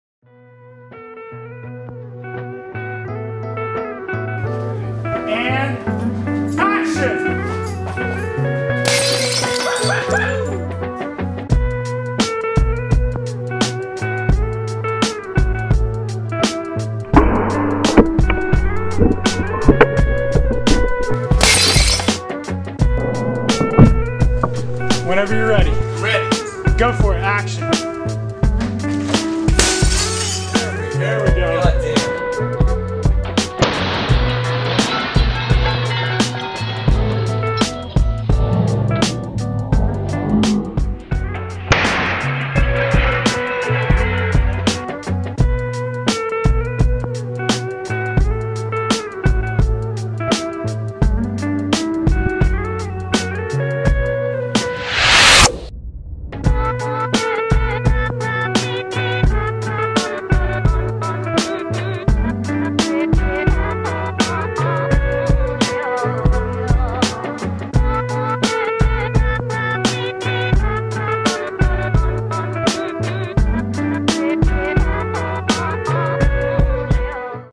SINK SMASH - Action Hero Behind the scenes
We smashed a TV and Sink for an effect in Action Hero
(short coming out 2017)